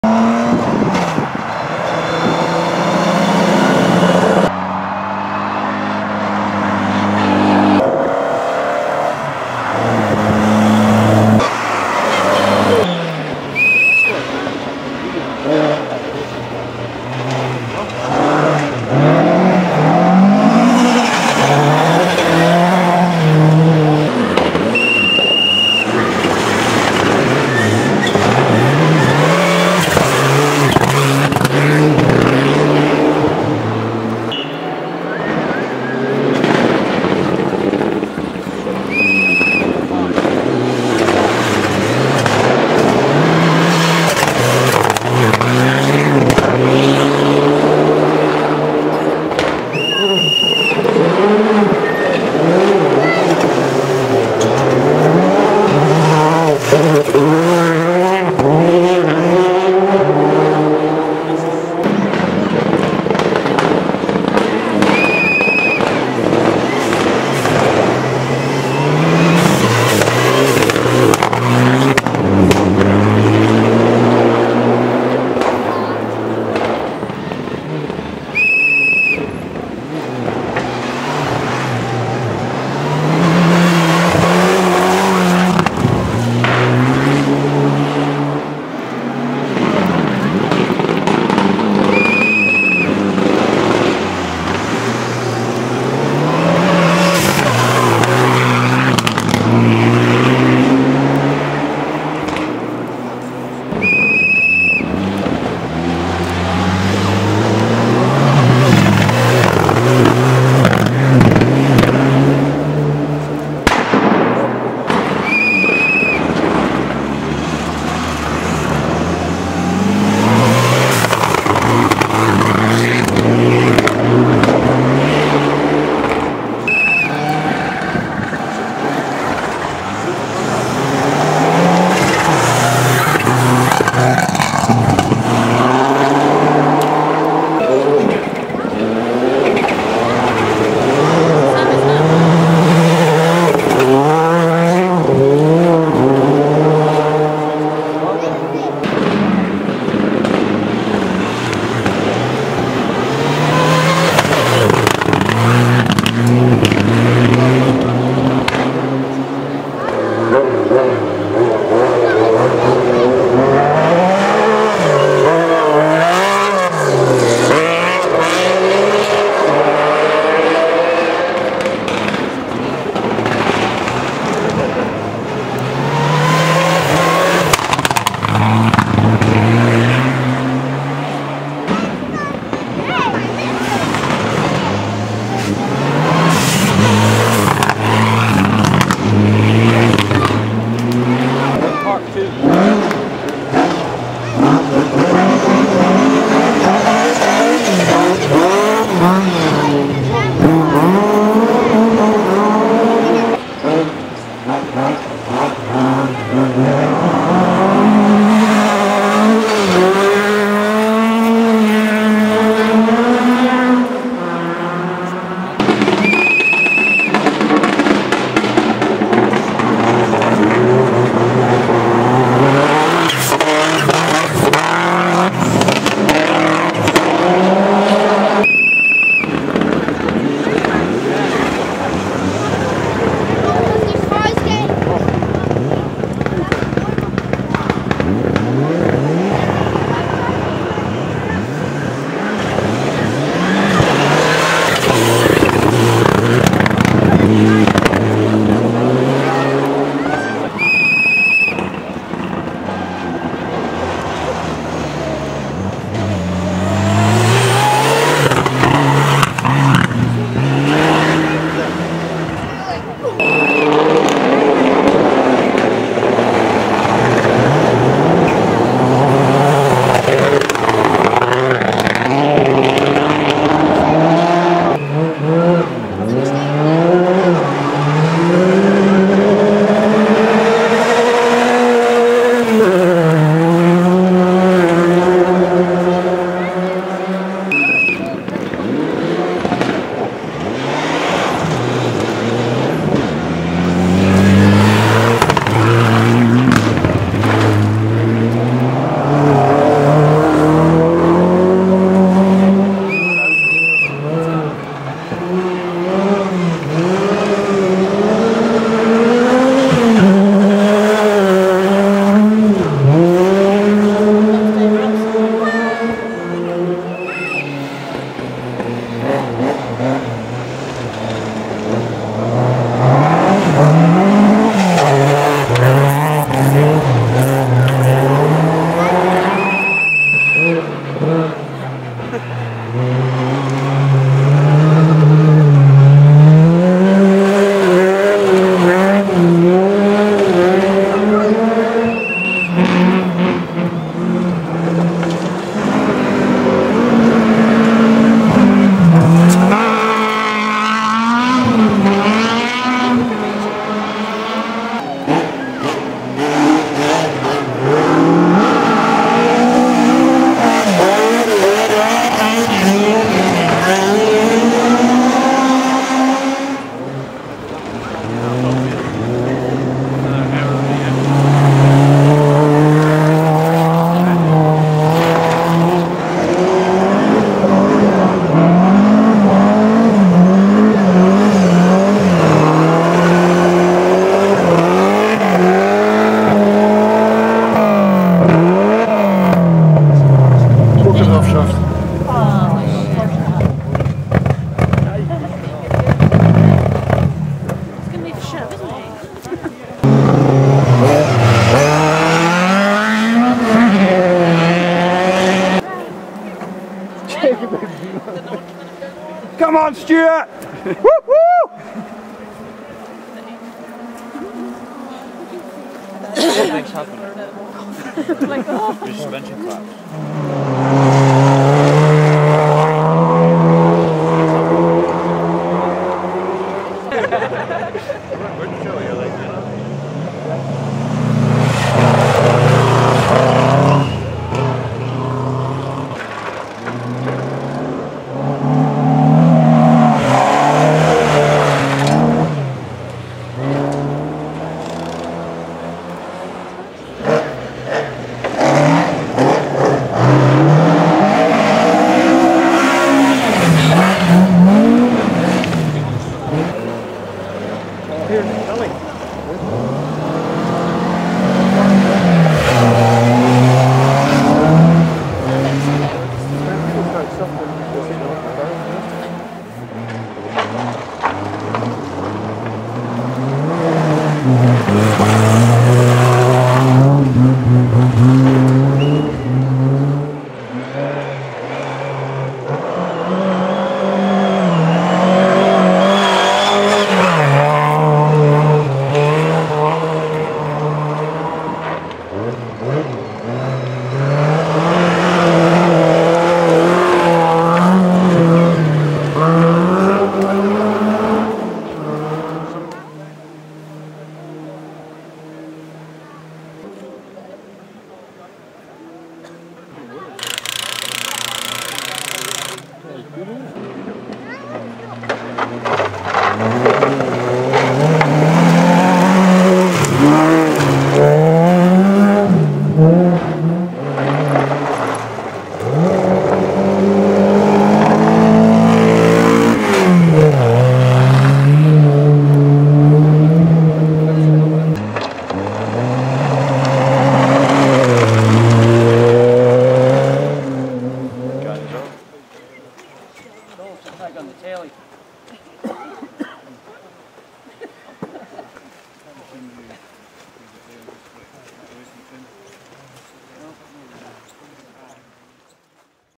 Snowman Rally 2014 Stage 5
Some cars heading to Stage 1 then views of Stage 5 at Scotsburn near Tain. We didn't get the best spot so spectators spoil the view a bit.